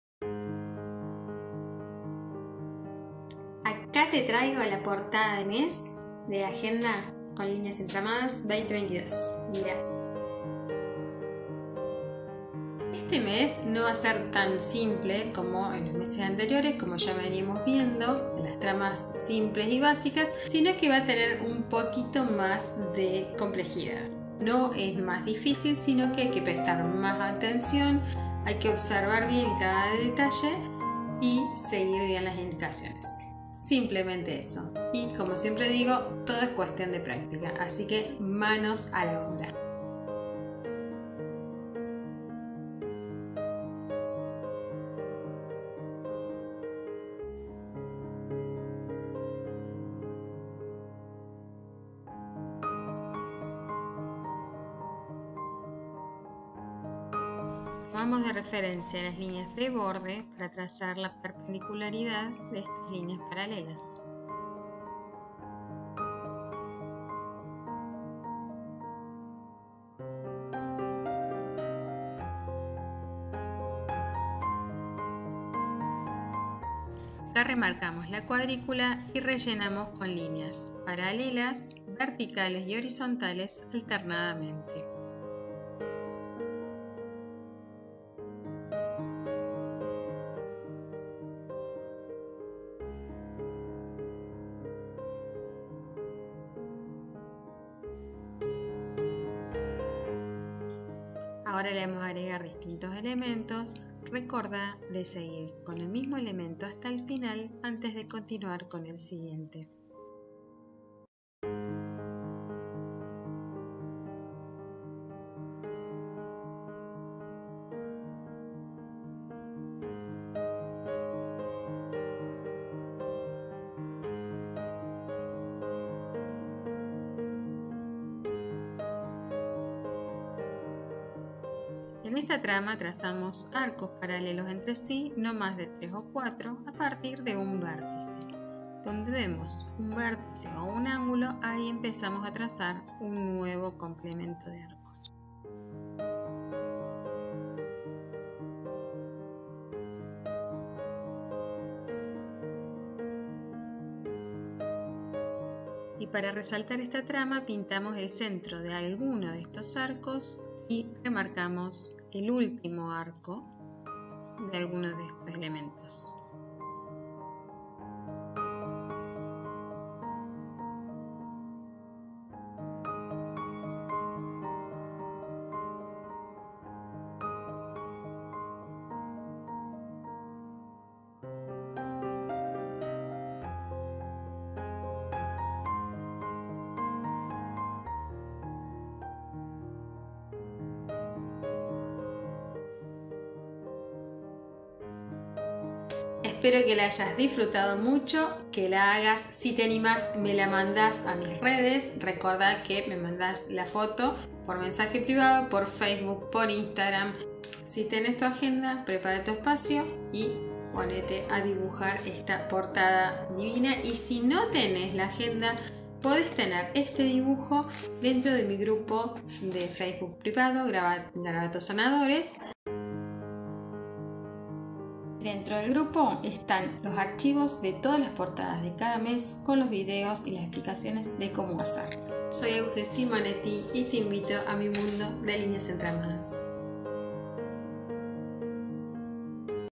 PORTADA DE MAYO
Acá te muestro como se hace la portada de MAYO, un poco más compleja que la de meses anteriores, para seguir aprendiendo tramas en esta primera parte del año.
Si ya tenés la agenda. arrancá
Si no la tenés... la podés adquirir en la tienda on line

Para tener el dibujo de la portada, puedes formar parte de mi grupo privado de facebook: "garabatos sanadores" te veo dentro!!

Hasta el mes que viene!!